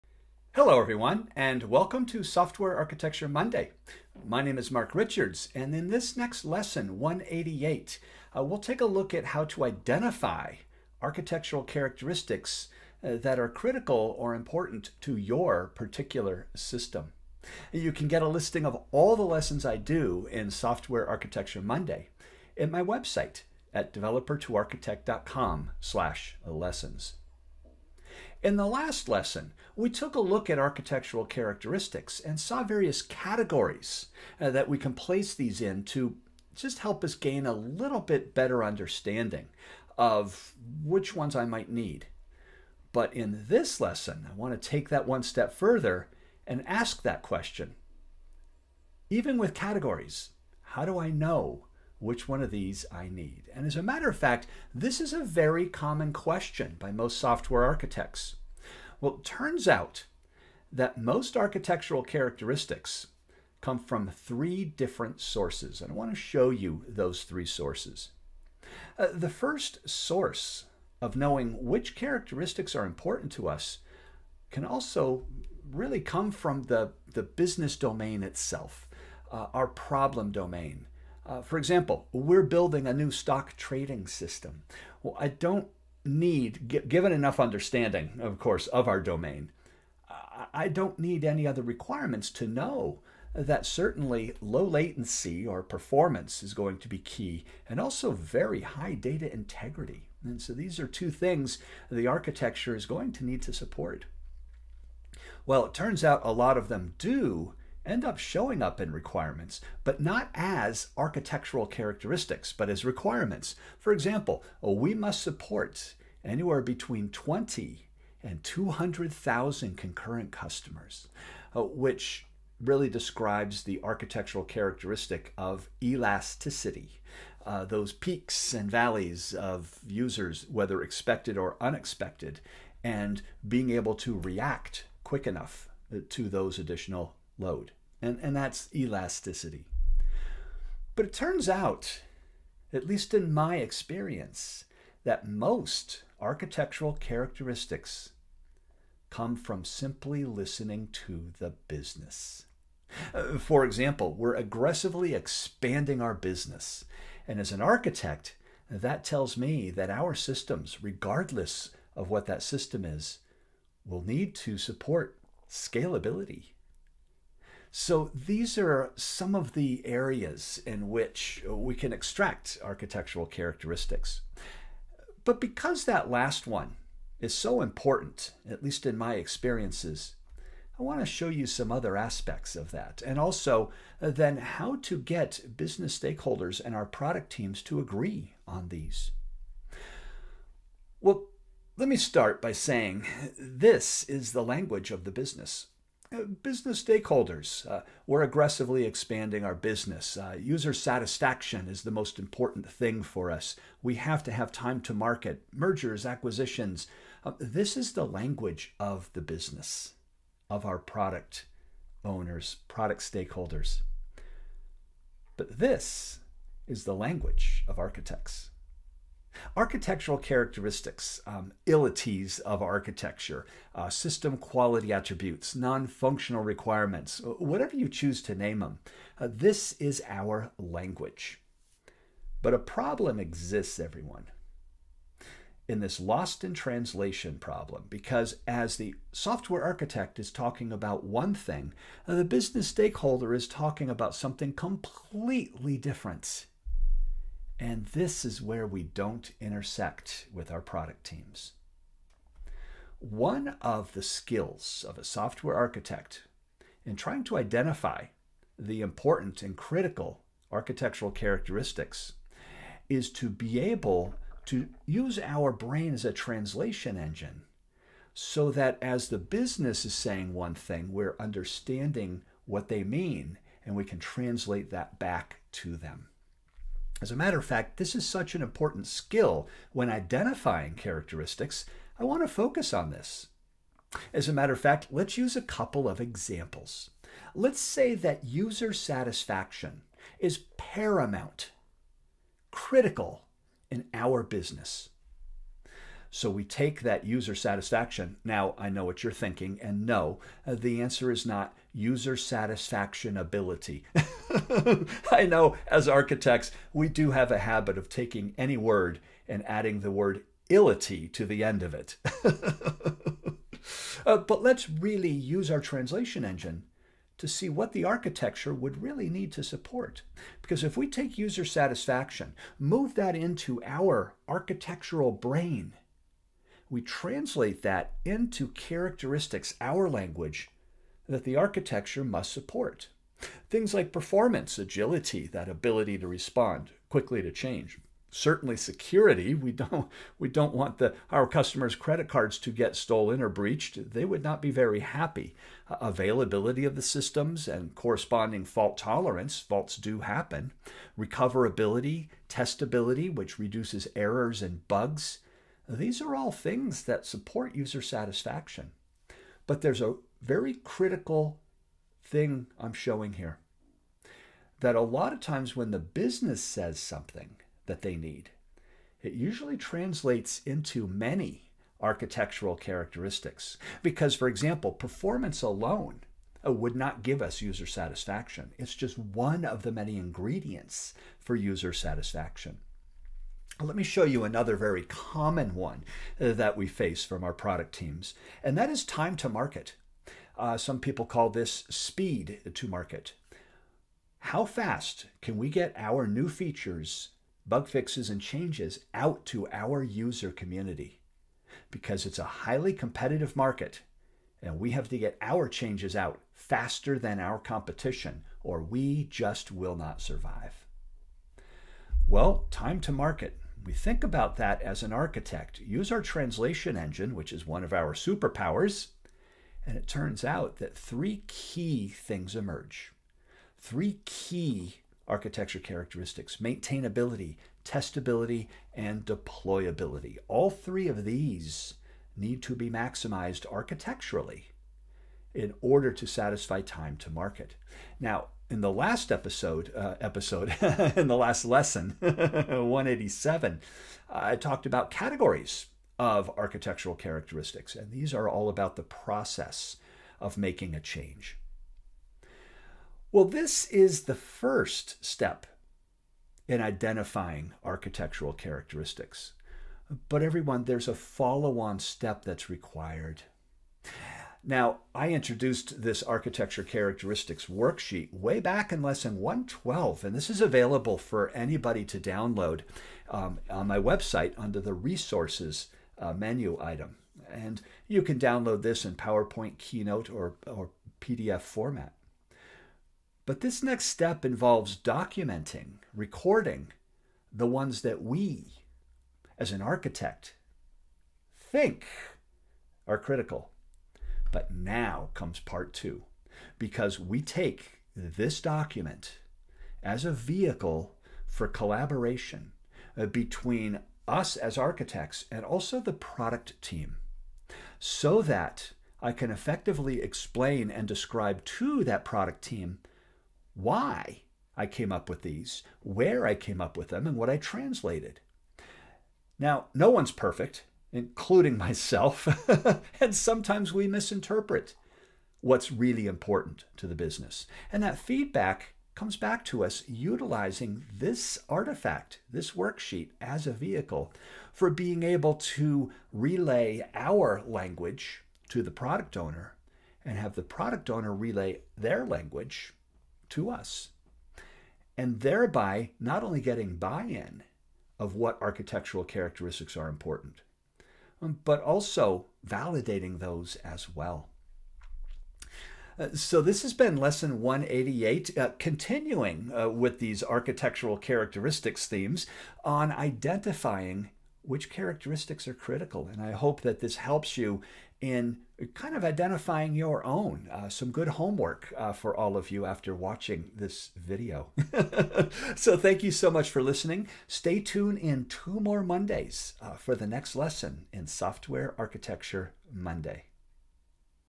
Lesson 188 - Identifying Architectural Characteristics
Out of the hundreds of architectural characteristics out there, how do you know which ones are important to your particular system? Is scalability critical? How about responsiveness, or availability, or even testability? In this lesson I discuss various techniques for identifying which architectural characteristics are important to your system, and how to communicate those to your product team and business stakeholders.

Fundamentals of Software Architecture
Software Architecture: The Hard Parts